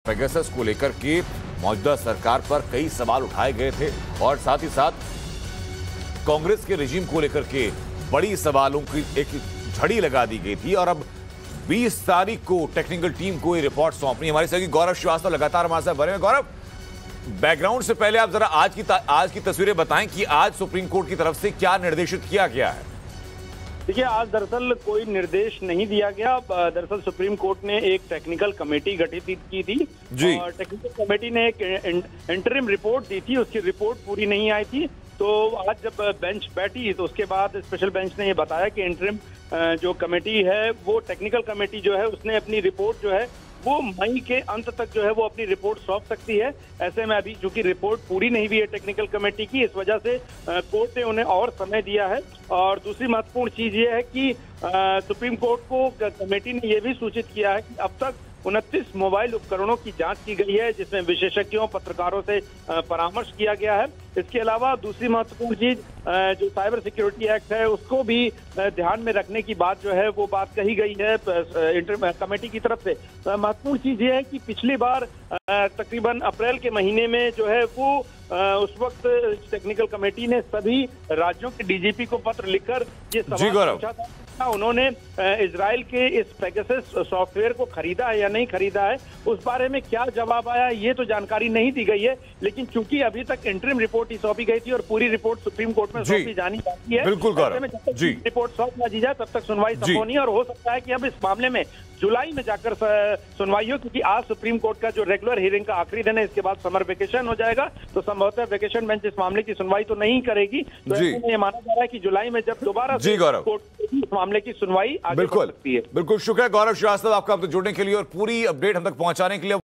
Pegasus मामले पर SC में सुनवाई आज, टेक्निकल टीम को रिपोर्ट दाखिल करने के लिए दिया 20 जुलाई तक का समय
Pegasus स्पाईवेयर मामले में आज सुप्रीम कोर्ट में सुनवाई जारी है | जिसके बाद कोर्ट ने टेक्निकल टीम को रिपोर्ट दाखिल करने के लिए ने 20 जुलाई तक समय दिया गया है, साथ ही अभी तक 29 मोबाइल फोन की जांच पूरी हुई है।